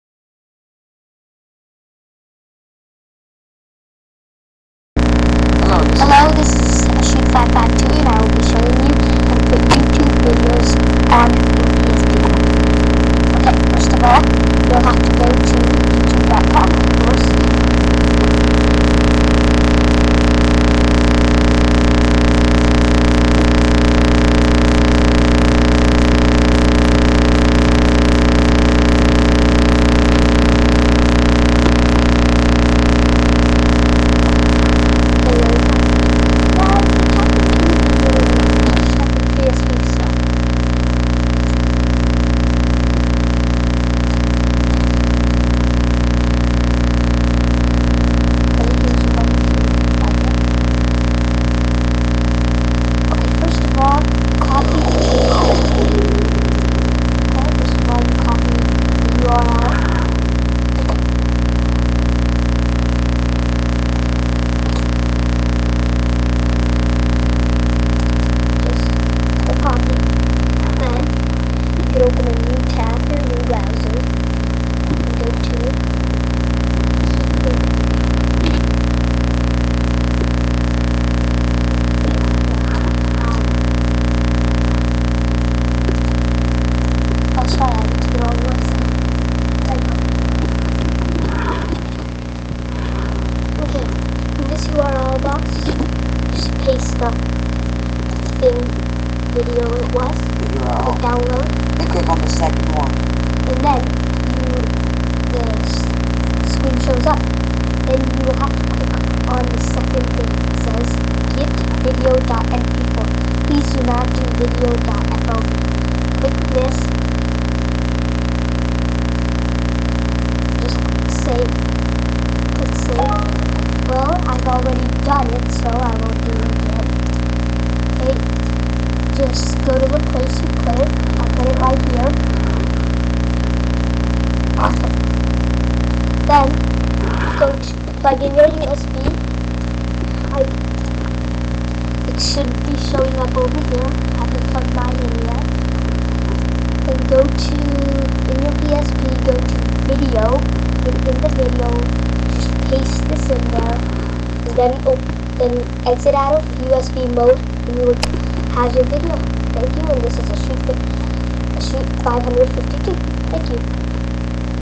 How to get cool youtube videos on psp
i will be showingyou how to download you tube videos to psp first go to youtube.com and select the video and copy it. the n go and paste in the url box and click download on the one with get_video.mp4. then plug usb and copy it to video and ur done plses subsrcibe